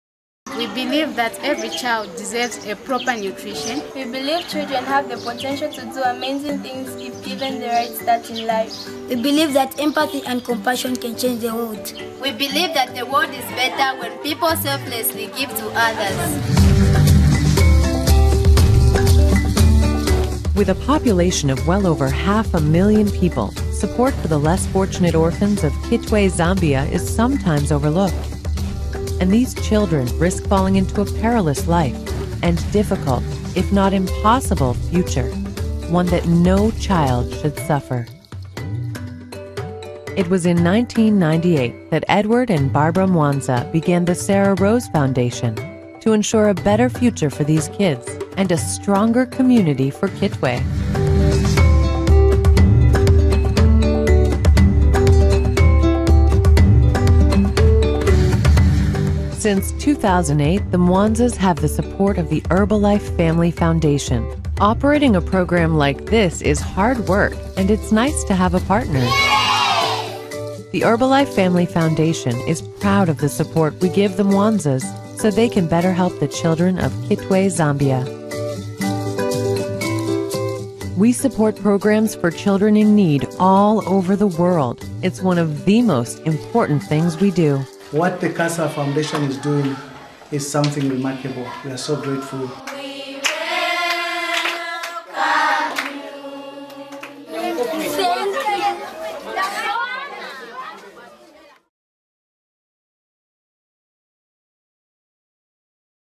CASA Herbalife with Sara Rose Children Foundation
Nutrition is the lifeline of children. Good nutrition is the centre of a happy and healthier life in Christ Jesus. A case of how Herbalife has impacted life through Sara Rose in the Kitwe rural communities they operate in.